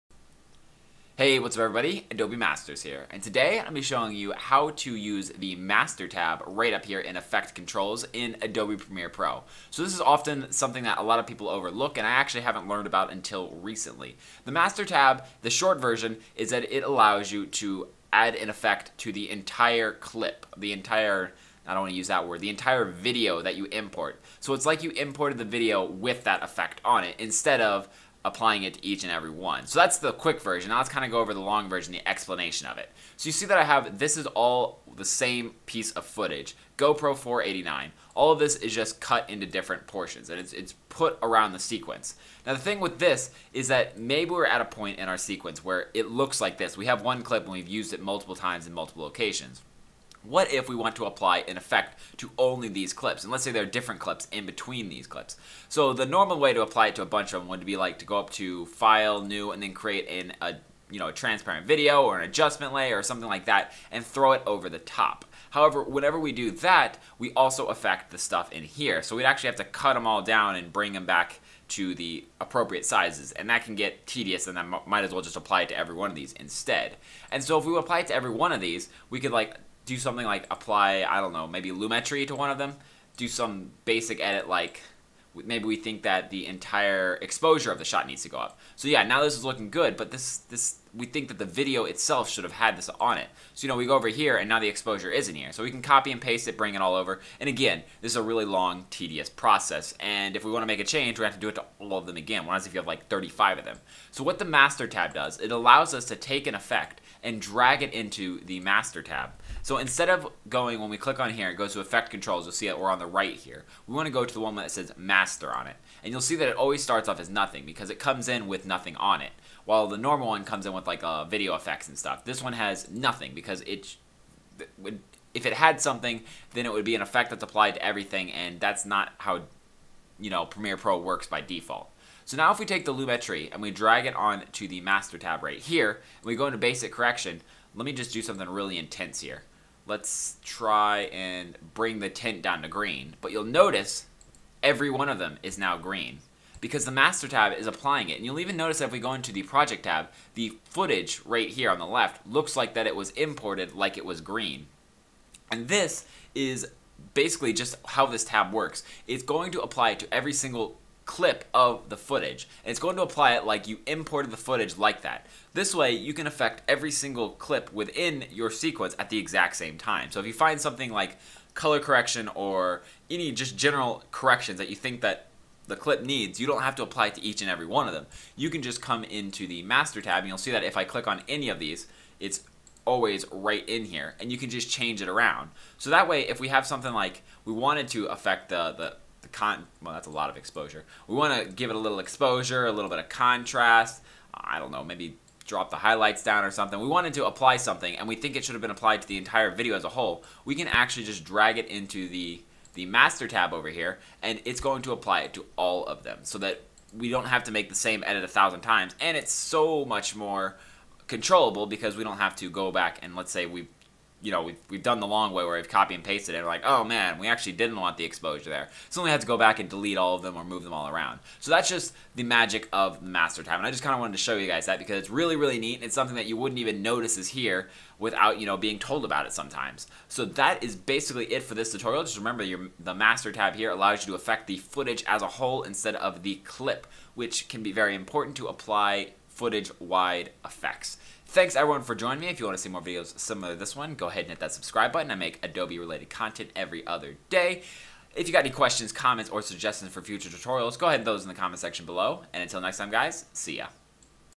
How to Use the Master Tab in Adobe Premiere Pro CC (2017)
Today I go over a little quick tip that allows you to edit the entire footage, instead of just clips from it.